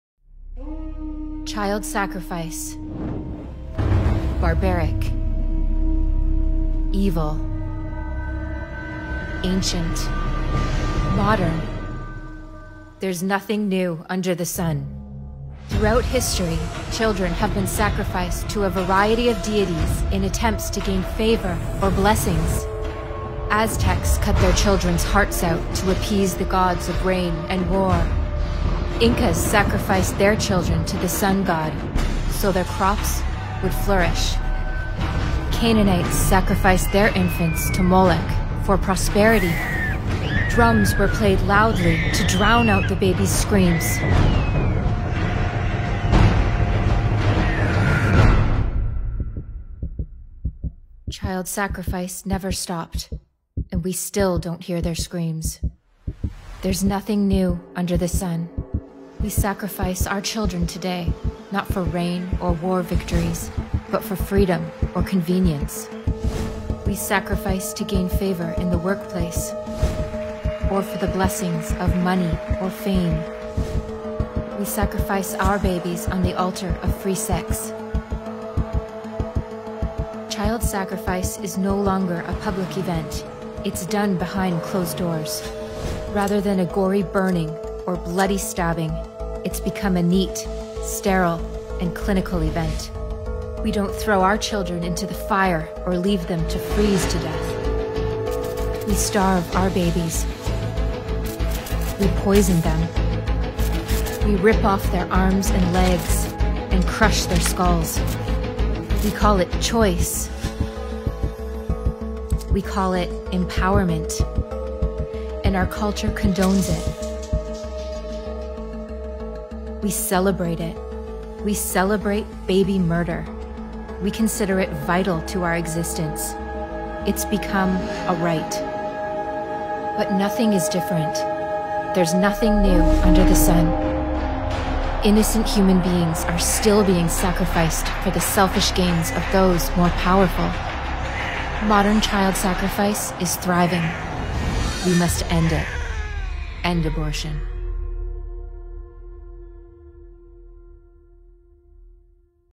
MODERN CHILD SACRIFICE
Do you think child sacrifice is a thing of the past?

Video from Choice42